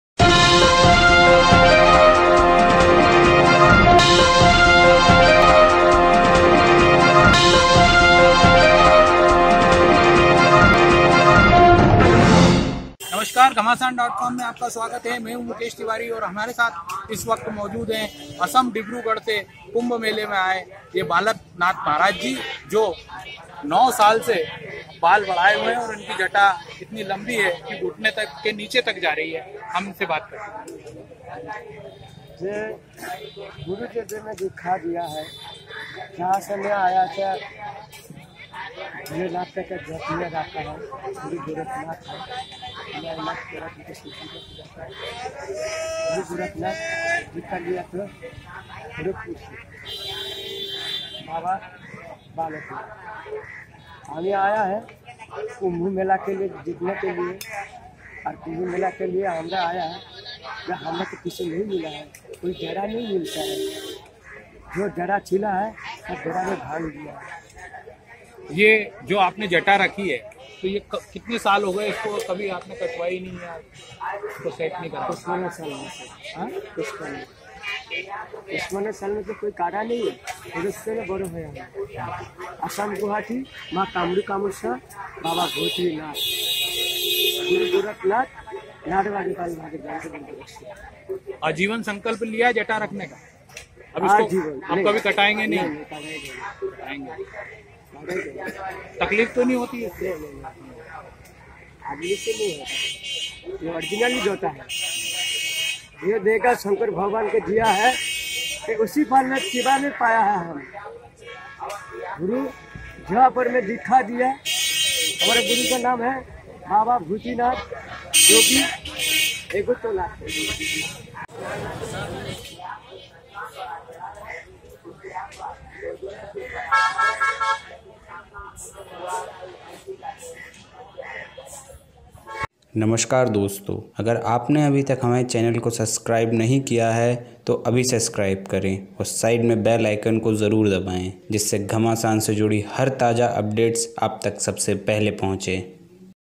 Prayagraj kumbh 2019 संत बालकनाथ से ख़ास बातचीत | Spiritual Sadhu Maharaj
पैर के पंजों को छूने ही वाली है इनकी जटा